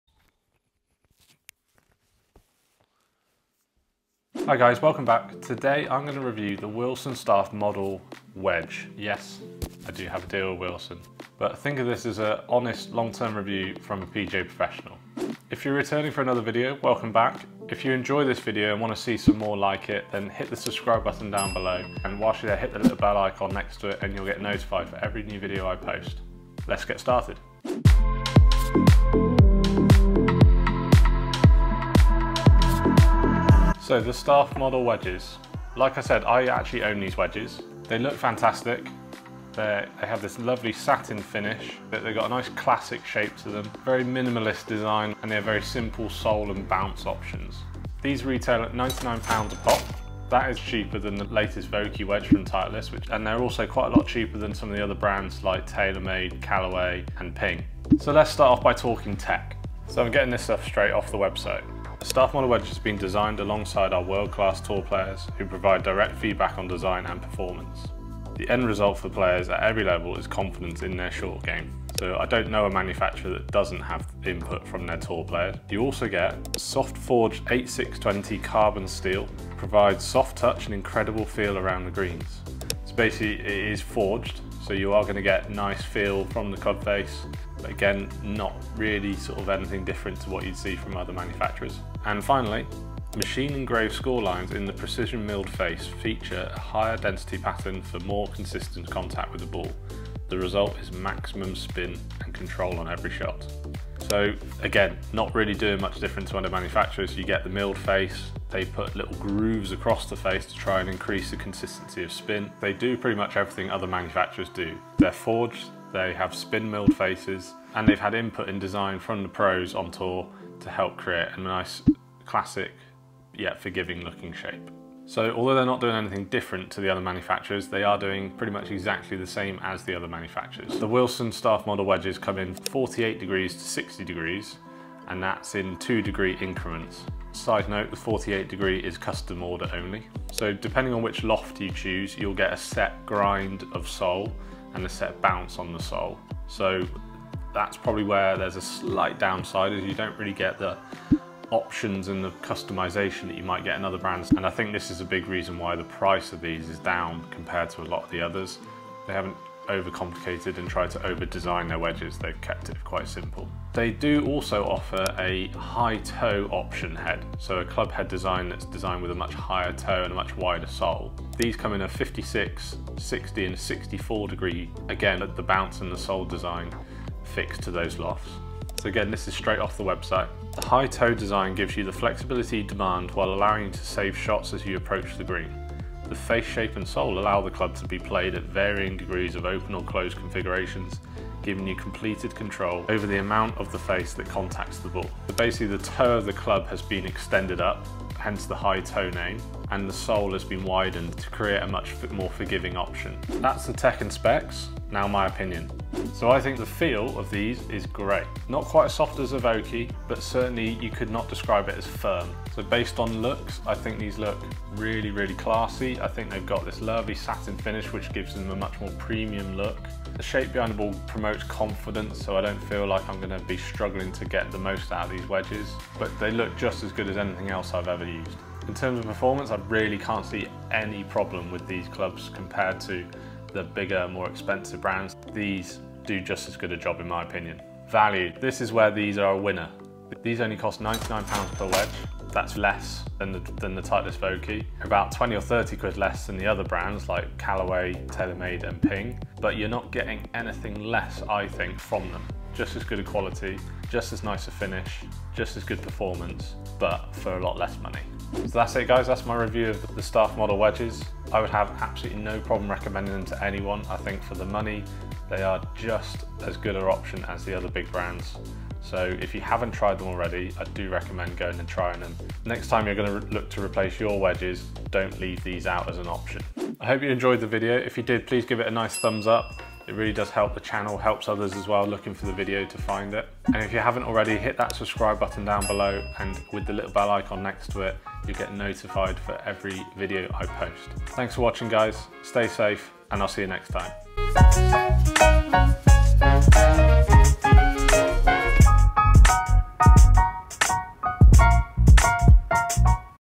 Wilson Staff Model Wedge Review | Best Wedges To Buy 2021?
This is my review of the Wilson Staff Model Wedges.
I own these wedges so here is my opinion on them!